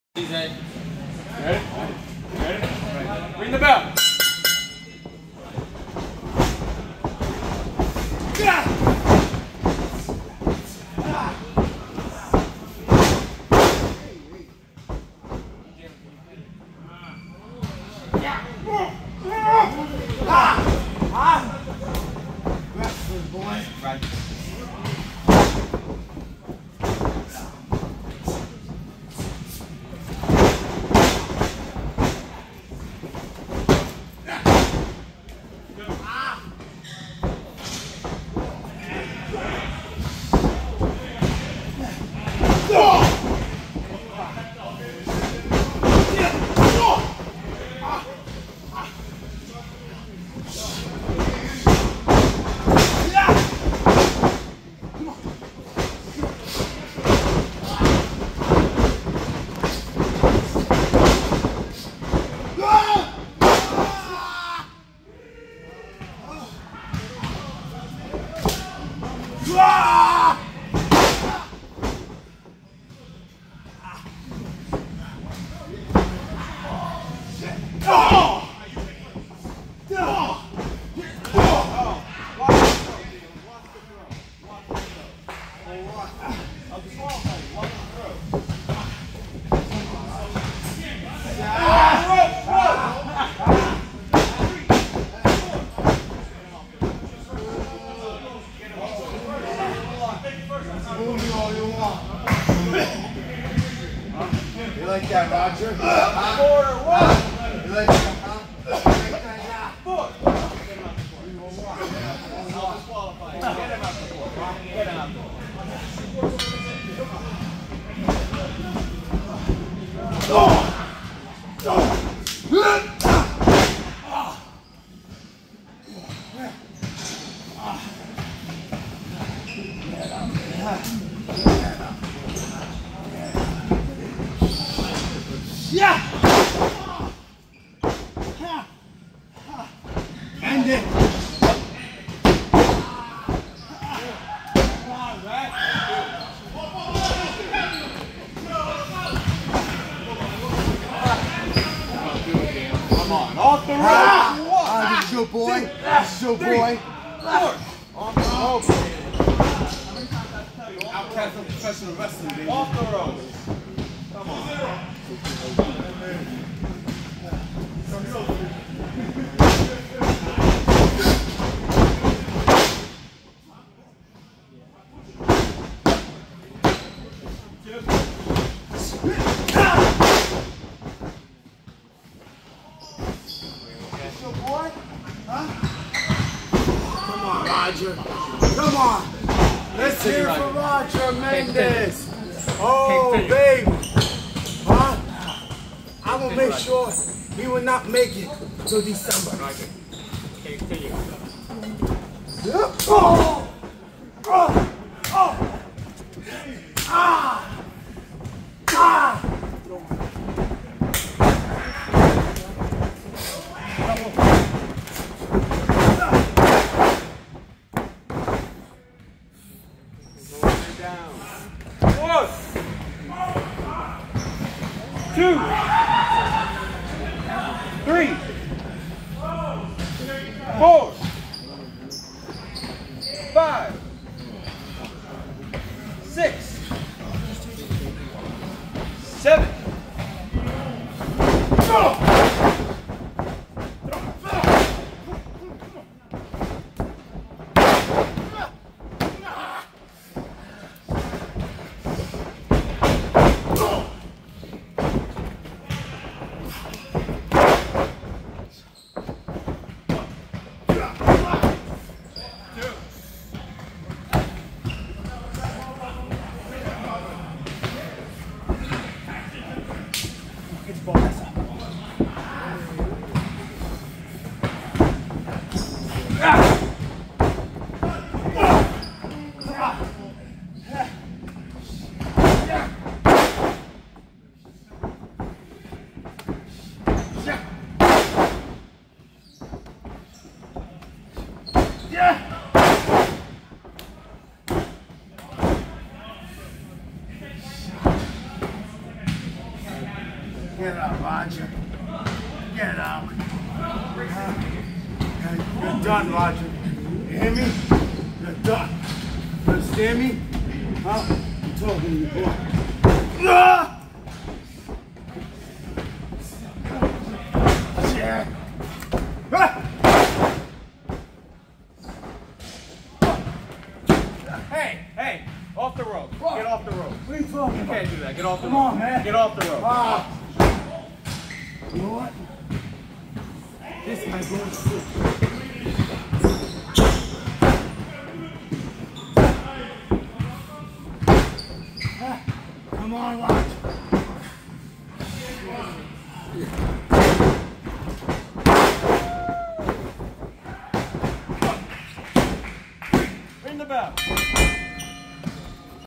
Warriors Showcase - Roger Mendez v Jalen White
This match is from the Warriors of Wrestling Training center located in Staten Island, NY...up and coming students from the training center and roster members square off in these unreleased matches. Roger Mendez v Jalen White.